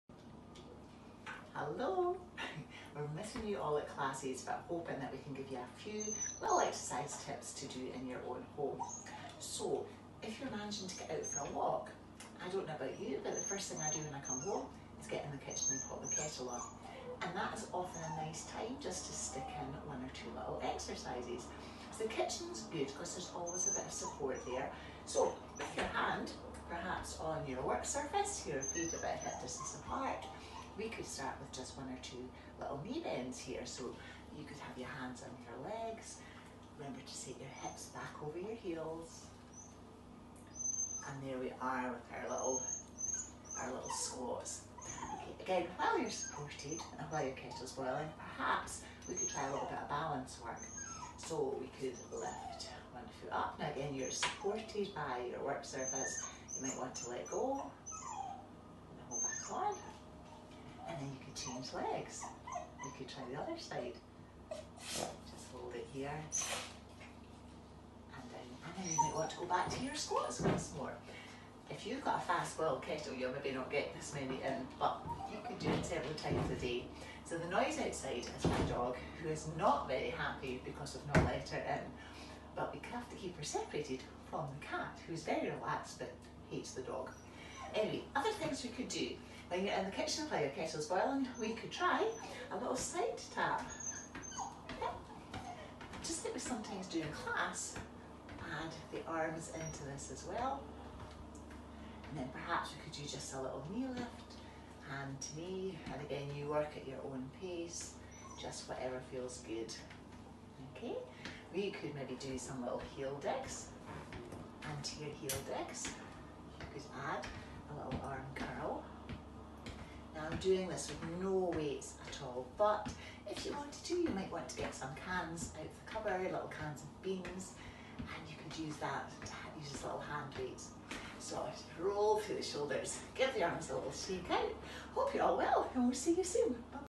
Exercise while the kettle boils
Our Group Fitness Instructor, Val Castles, takes us through a 3-minute standing routine. This is a simple session that you can do while any time of the day